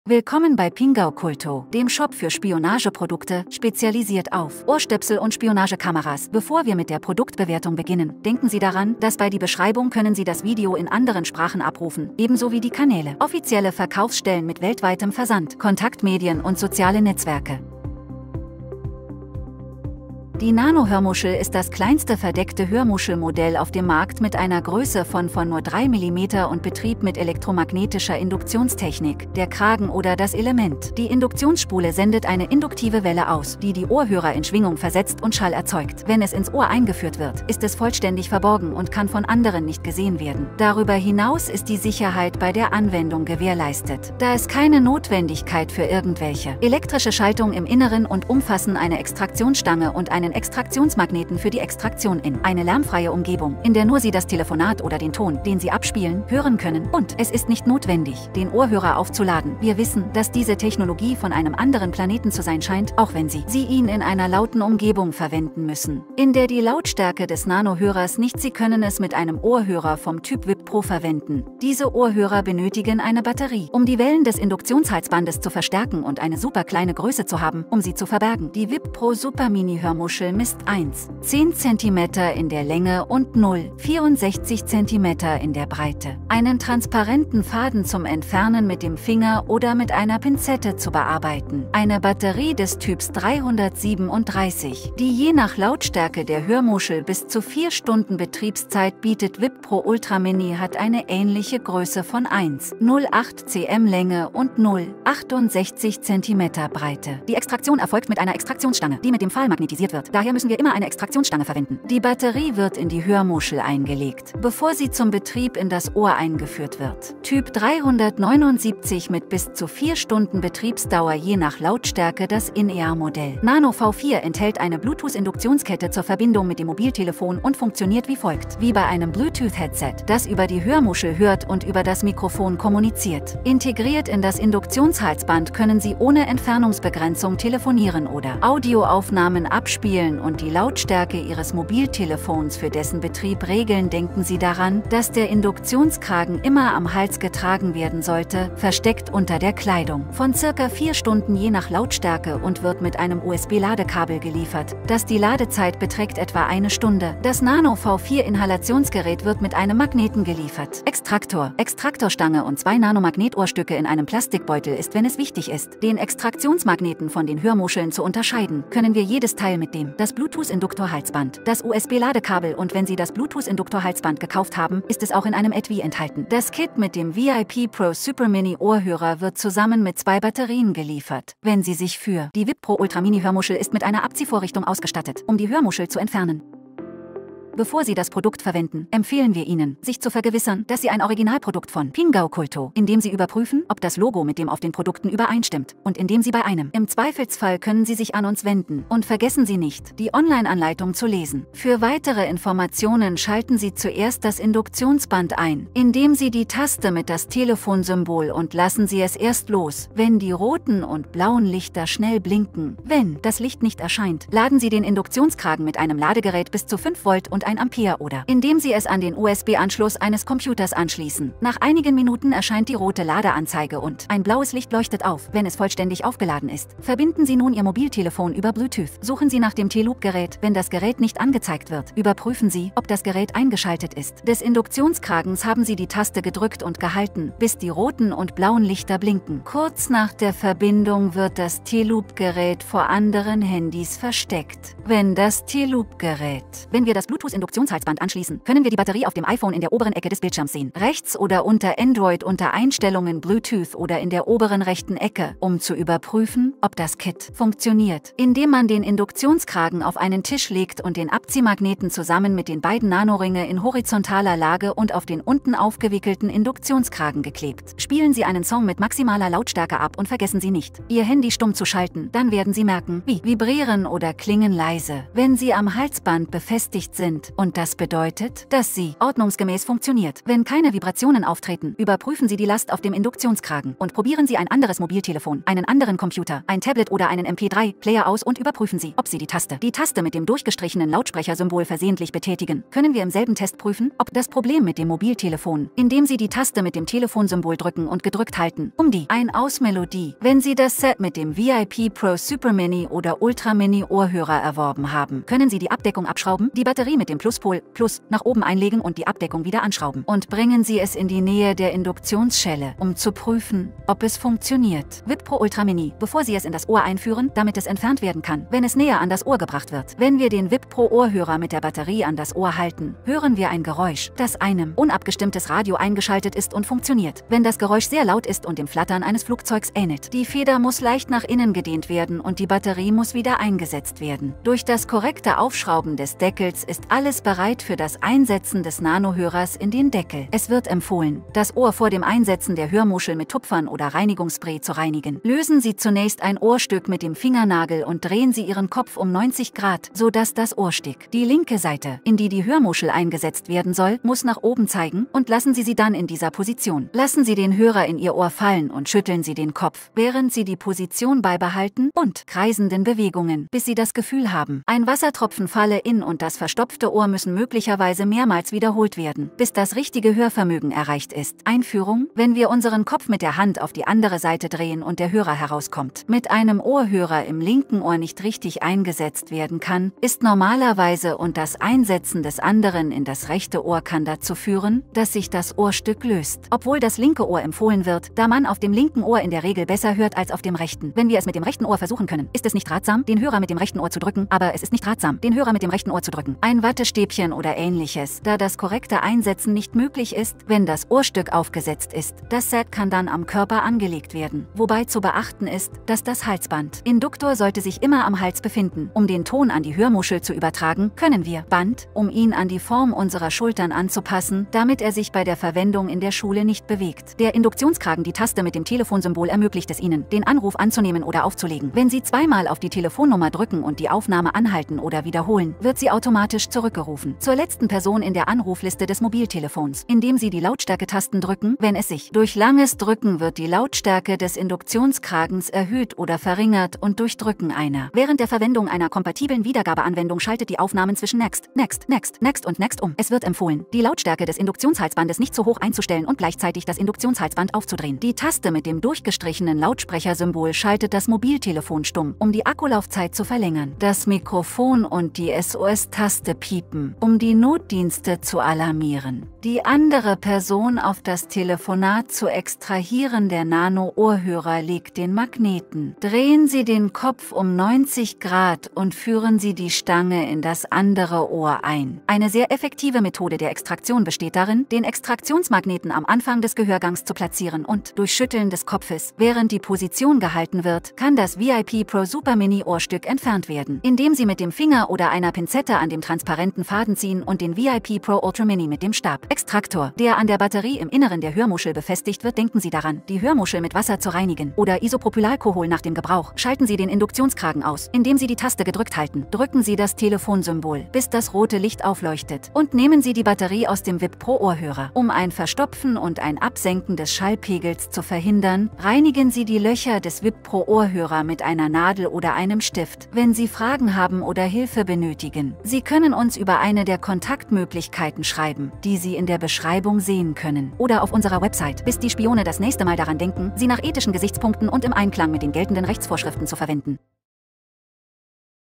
Unsichtbare Kopfhörer Nano V4 - Rückblick und Tutorial - Deutsch | PingaOculto
🌍 Denken Sie daran, dass Sie das Video in anderen Sprachen ansehen können 🌎

🛒 Kauflinks 🛍️
- Geschäft in Madrid (Spanien): Calle Lucio del Valle 12, 28003, Madrid

📞 Kontaktmöglichkeiten 🟢.

00:00 EINLEITUNG
00:20 ÜBERSICHT
02:38 TUTORIAL

In diesem Video stellen wir Ihnen das Unsichtbare Kopfhörer Nano vor, ein revolutionäres Gerät, mit dem Sie effektiv und diskret mit einer Größe von nur 3 Millimetern kommunizieren können. Außerdem zeigen wir Ihnen den Inhalt des Produkts und führen Sie durch ein praktisches Video-Tutorial, wie Sie es richtig benutzen.

🔍 Testbericht und Eigenschaften

In diesem ausführlichen Testbericht gehen wir auf die wichtigsten Eigenschaften des Unsichtbare Kopfhörer Nano ein. Sie werden sein kompaktes Design entdecken, das ideal ist, um unbemerkt zu bleiben. Wir werden auch seine fortschrittliche Technologie untersuchen, die eine stabile und sichere drahtlose Verbindung durch elektromagnetische Induktion ermöglicht.

📦 Inhalt und mitgeliefertes Zubehör

Bei den Produktinhalten zeigen wir Ihnen alles, was Sie beim Kauf des Unsichtbare Kopfhörer Nano erhalten werden. Angefangen von der versteckten Hörmuschel selbst bis hin zu weiterem Zubehör, wie der Induktionsschleife und den für die Nutzung benötigten Adaptern oder Geräten.

📹 Video-Anleitung

Im Video-Tutorial führen wir Sie durch alle notwendigen Schritte, um den Unsichtbare Kopfhörer Nano effektiv einzurichten und zu nutzen. Sie erfahren, wie Sie ihn an Ihr Mobilgerät anschließen, die Lautstärke einstellen und alle Funktionen optimal nutzen können.

Sind Sie bereit, Ihre Kommunikation auf diskrete und effiziente Weise zu verbessern? Verpassen Sie nicht dieses Video über den Kopfhörer Nano.

Vergessen Sie nicht, Ihre Kommentare und Fragen im Kommentarbereich zu hinterlassen. Wir sind hier, um alle Ihre Fragen zu beantworten und danken Ihnen für Ihre Unterstützung 😊!